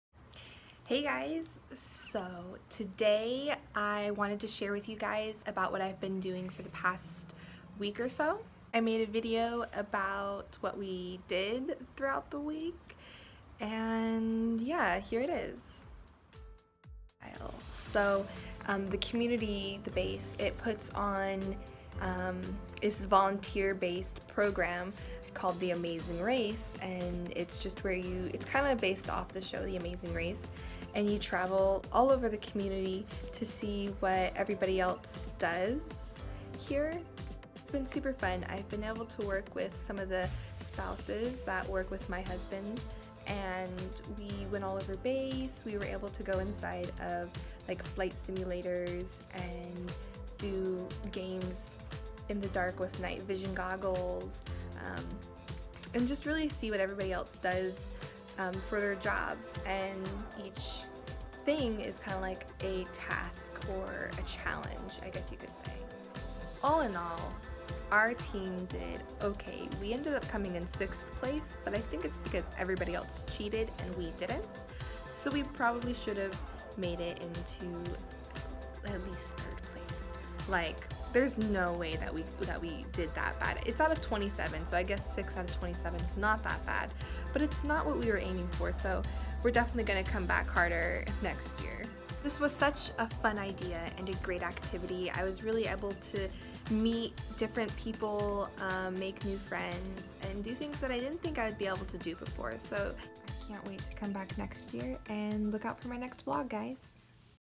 I did the Amazing Race!
This past week has been crazy busy and crazy fun! I have been able to hang out with some amazing people and participate in really fun activities around my community. The community puts on a volunteer-based activity called amazing race (which is A smaller version of the amazing race show). We do a bunch of challenges for three days in a row that are based off of jobs that people around the community do. This includes flight simulators, night vision goggles, running with heavy objects, and mental activities. It’s all a competition and you get points for each activity on how are you do. We ended up coming in sixth place out of 27… Which is good but we were aiming for at least top three! That just means we’re gonna come back harder next year!